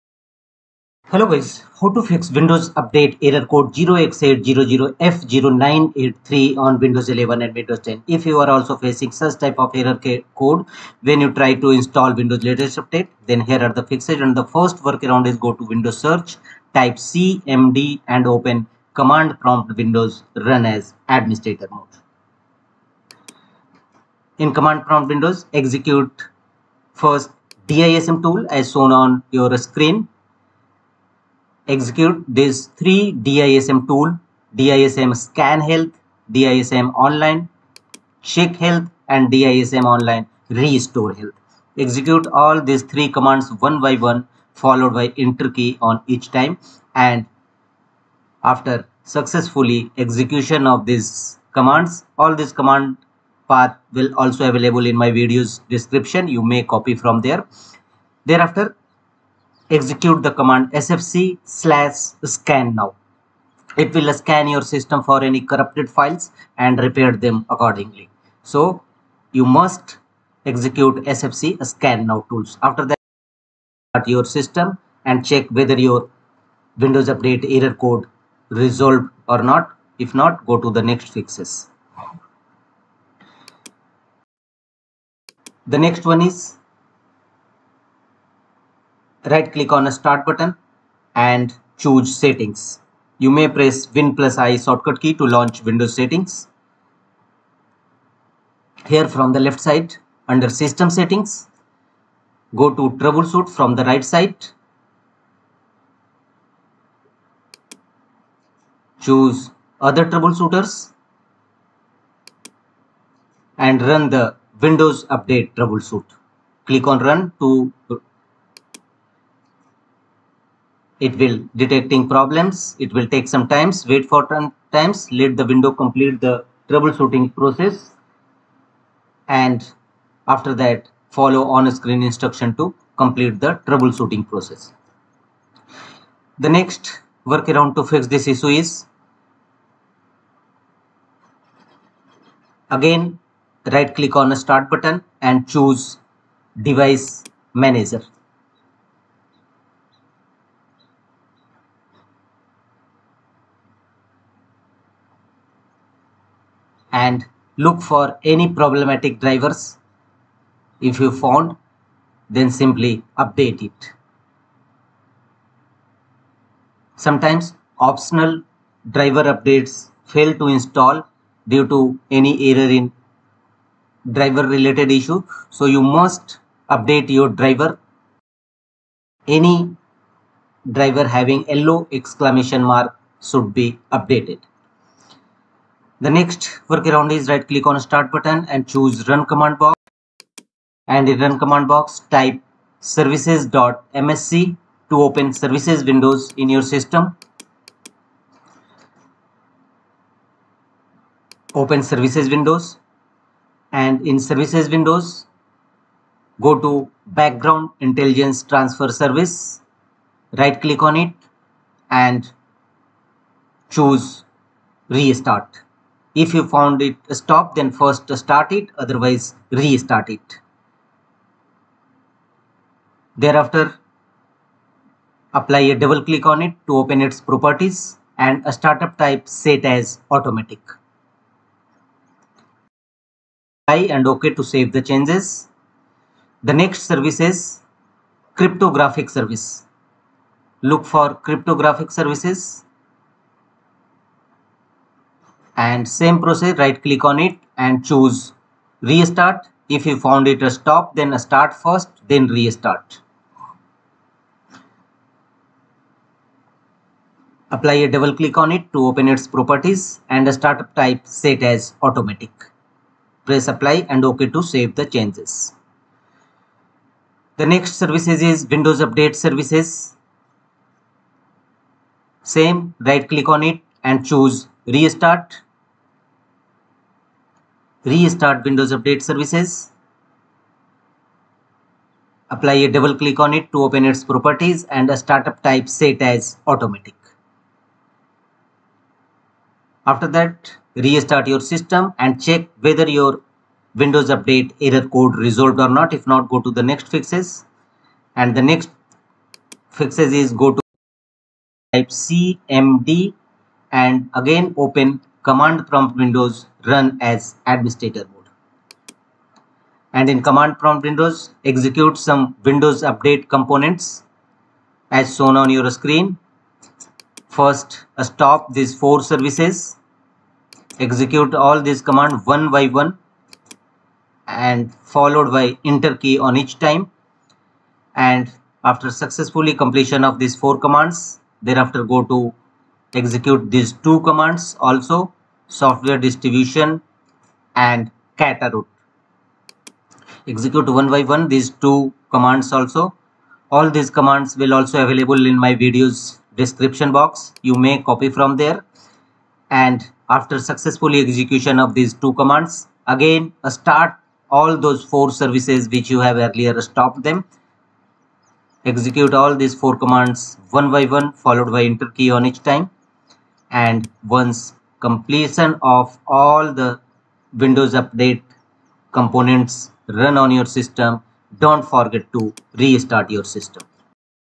Windows update error Code 0x800f0983 on Windows 11 / 10 Fixed
How to Fix Windows update error Code 0x800f0983 on Windows 11 / 10

Fix error